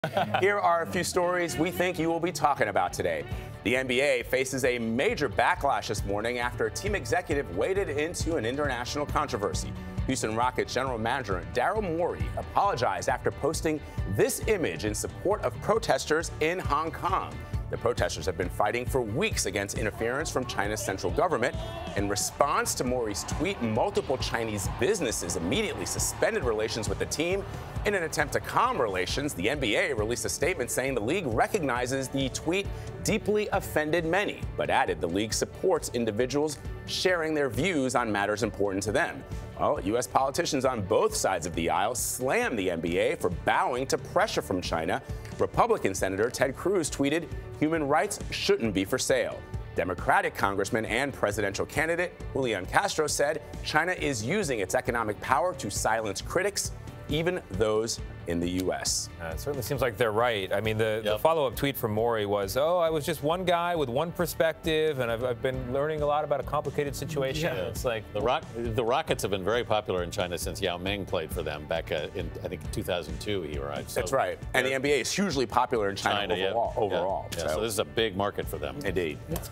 Houston Rockets GM ignites scandal with tweet backing Hong Kong protesters
The NBA faces major backlash after a team executive waded into an international controversy. Houston Rockets General Manager Daryl Morey apologized after posting this image in support of protesters in Hong Kong. The protesters have been fighting for weeks against interference from China's central government. In response to Morey's tweet, multiple Chinese businesses immediately suspended relations with the team.